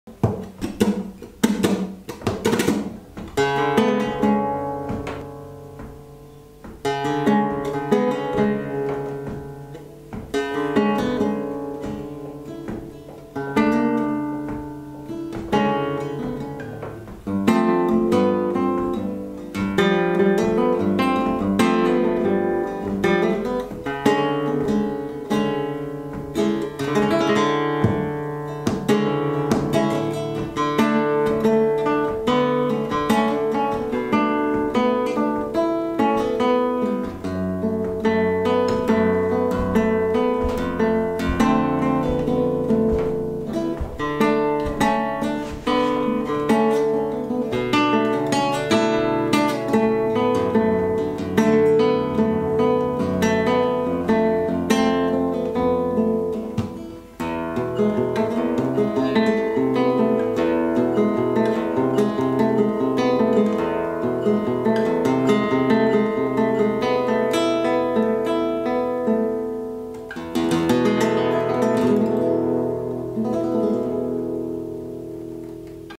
Rafael Romero 2E negra Spruce/Rosewood
Test run on a Rafael Romero Negra 2016 San Fernando, Cadiz